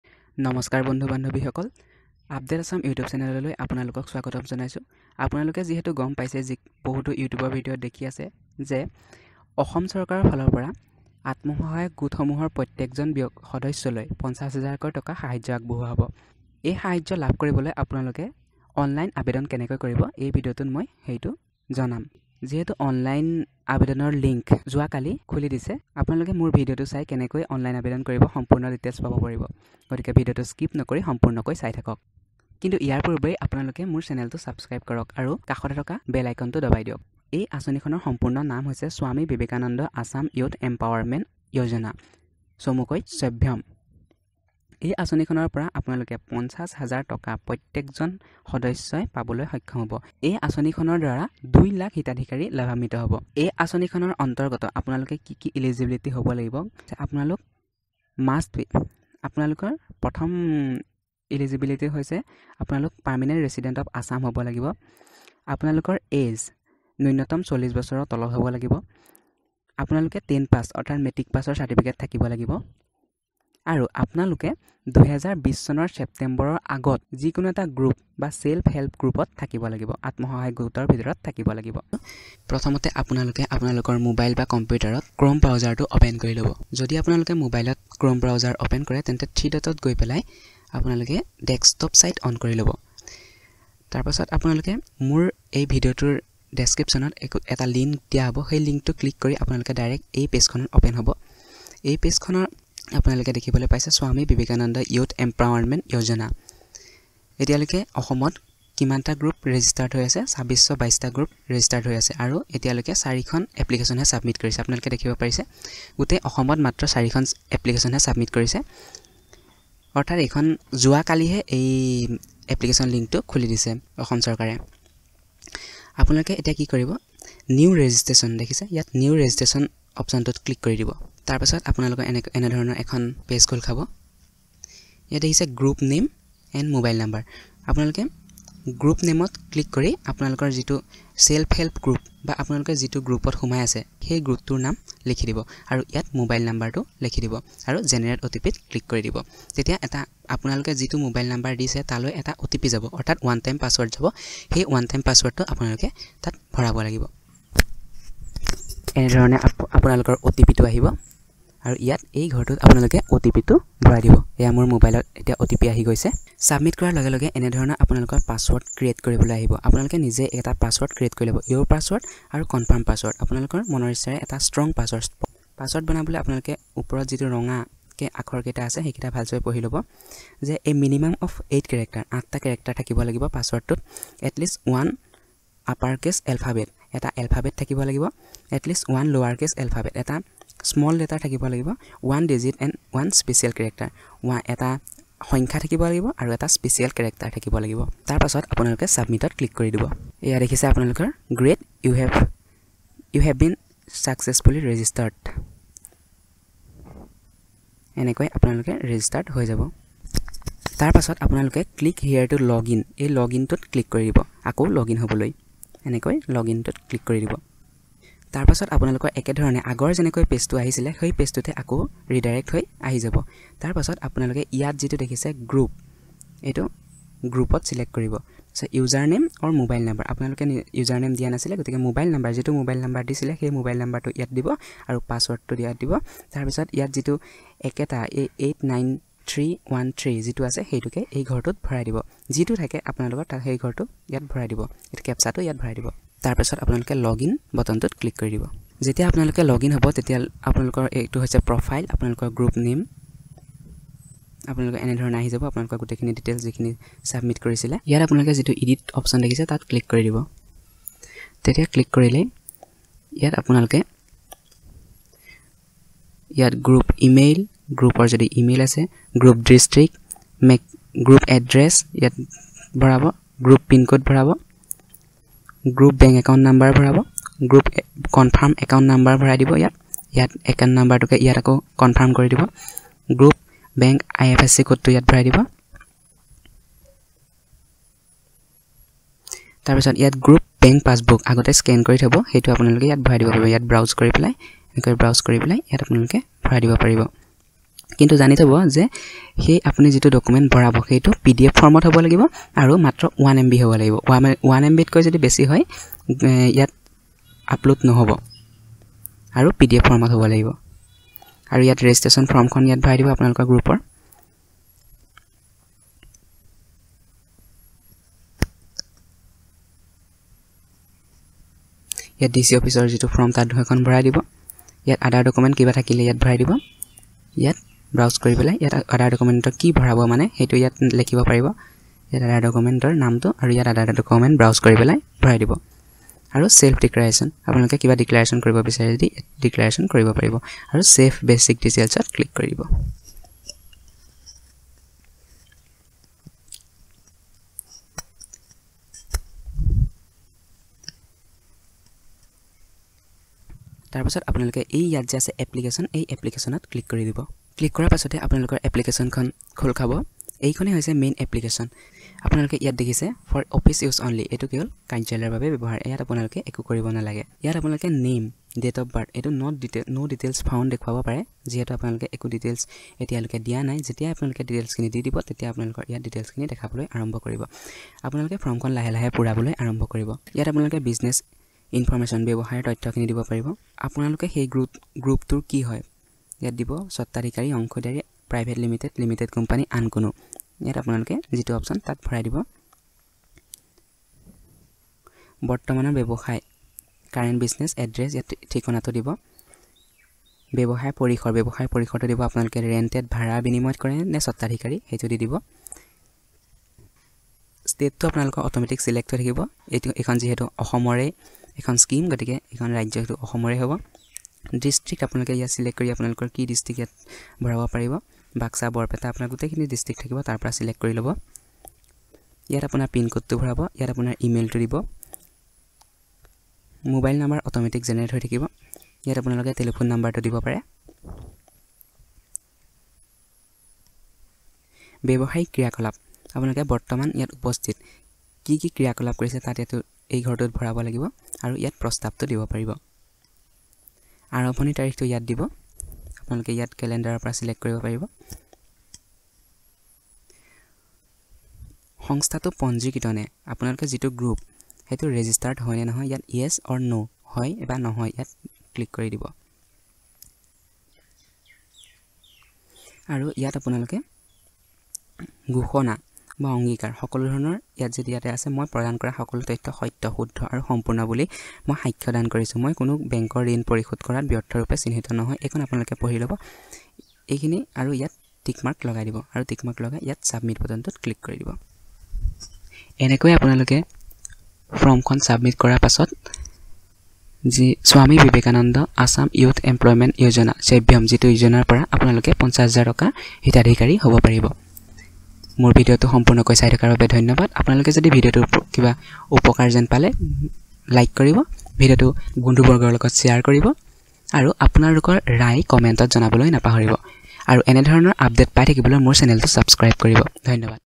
আত্মসহায়ক গোটৰ প্ৰত্যেক সদস্যৰ বাবে ৫০হাজাৰ টকা সাহায্য অসম চৰকাৰৰ SVAYEM Online Apply 2020
Hello friends.

Under this Re-SVAYEM scheme 2 lakhs educated youth of Assam will receive Rs.50,000/- .Swami Vivekananda Assam Youth Empowerment (SVAYEM) Scheme announced by the Hon’ble Finance Minister of Assam in his budget speech on 7th February, 2017.

View this best product :-

My Mic

My laptop

My Laptop Bag